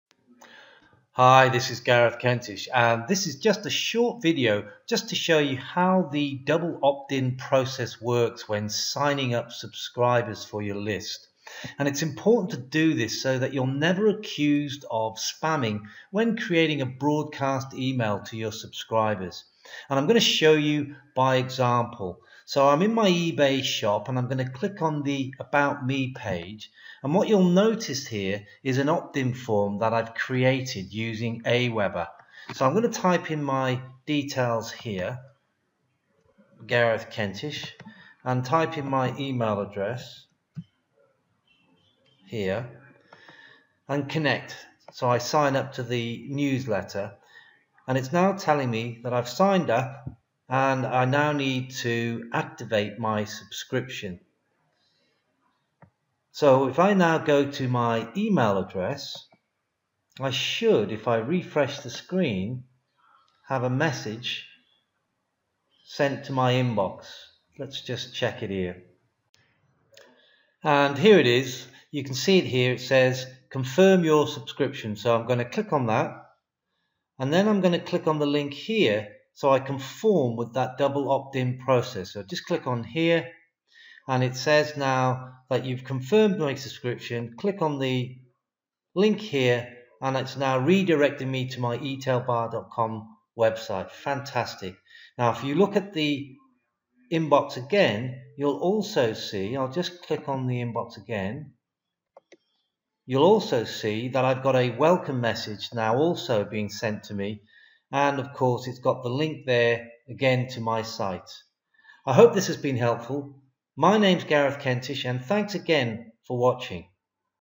Seeing how the double opt-in process works when building the LIST
So important to use the double opt-in process when building a subscriber list.
This video shows you how it works.
Gareth